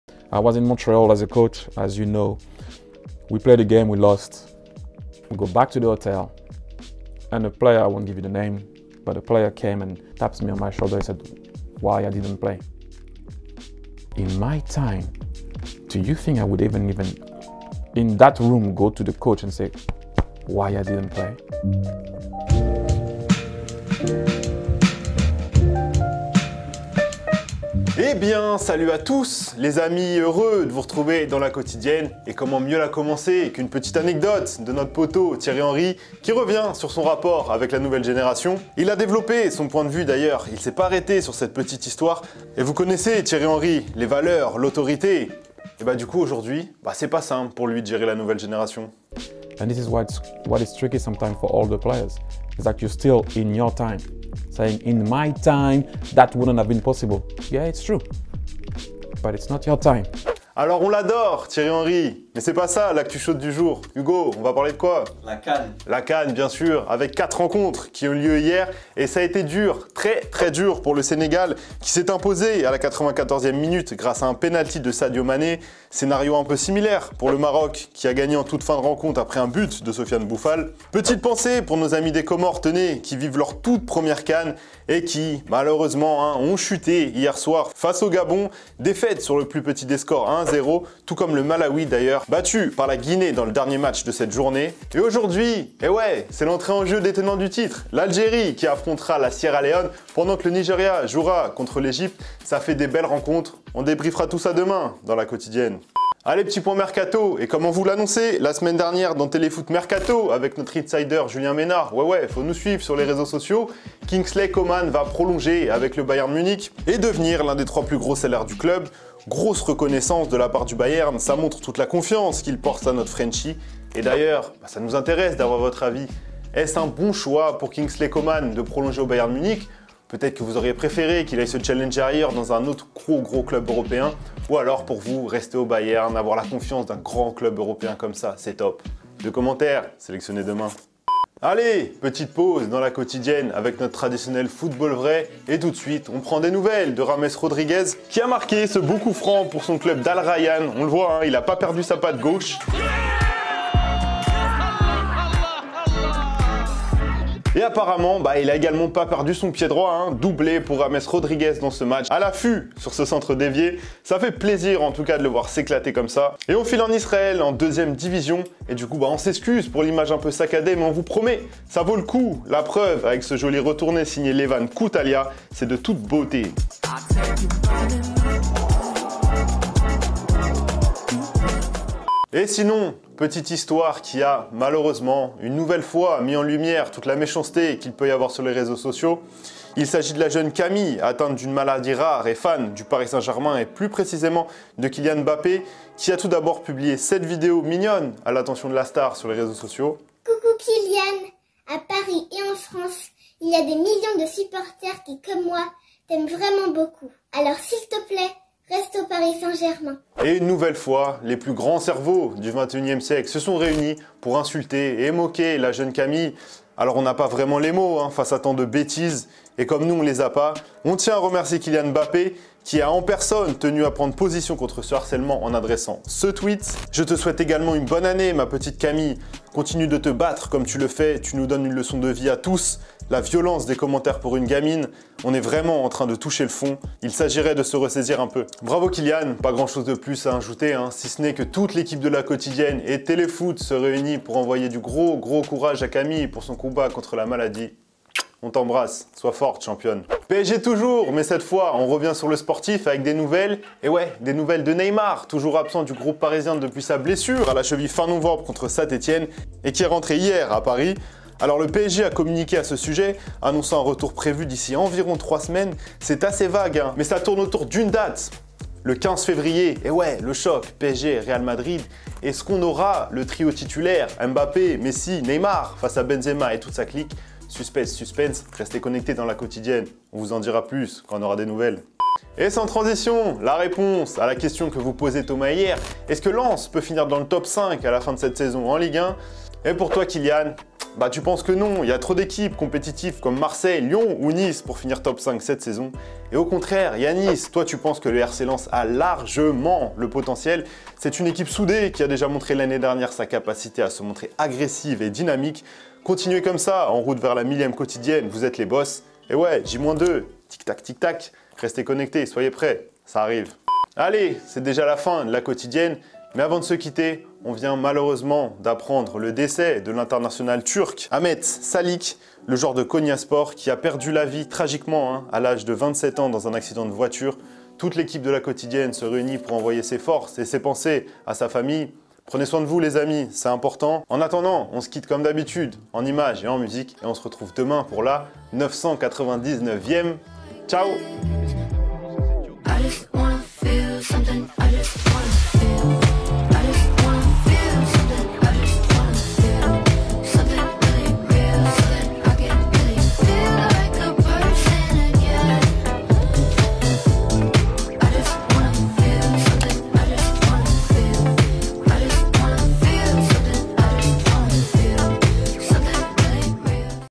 Mbappé VOLE AU SECOURS d'une enfant harcelée sur Twitter ! - La Quotidienne #998
Mbappé prend la défense d'une enfant harcelée sur Twitter, Coman va prolonger avec le Bayern Munich, le point sur le début de la CAN : la Quotidienne du jour est disponible !